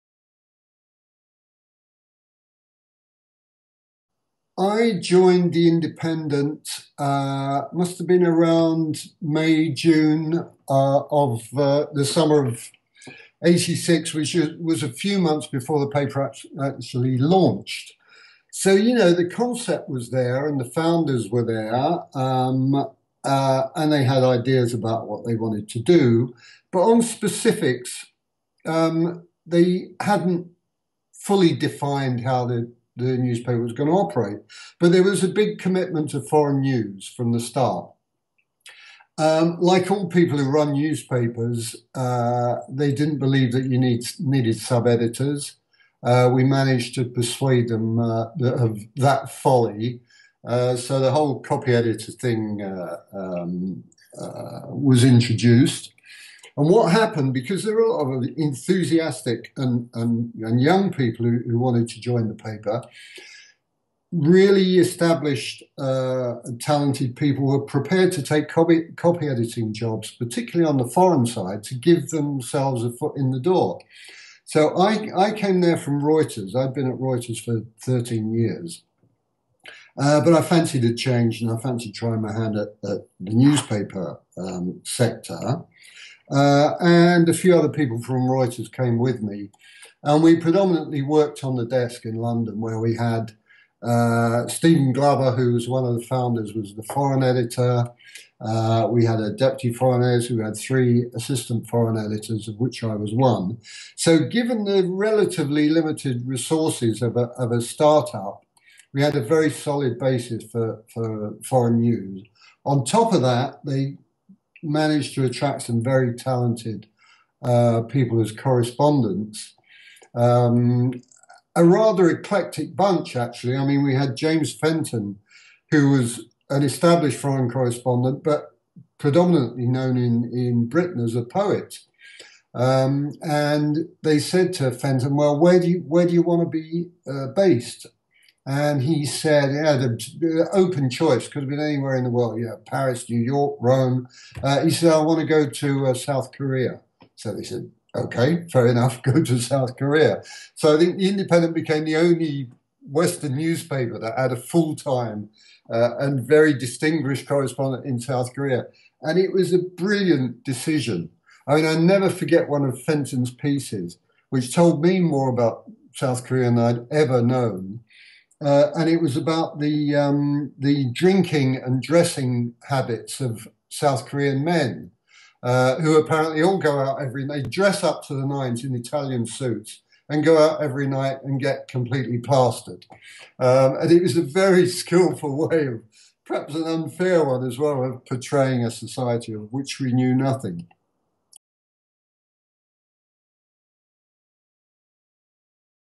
Wise investments in foreign news at the beginning
Harvey Morris explains that The Independent established a strong foreign news department from the very beginning, making a series of wise personnel investments including taking on poet James Fenton as a foreign correspondent in South Korea.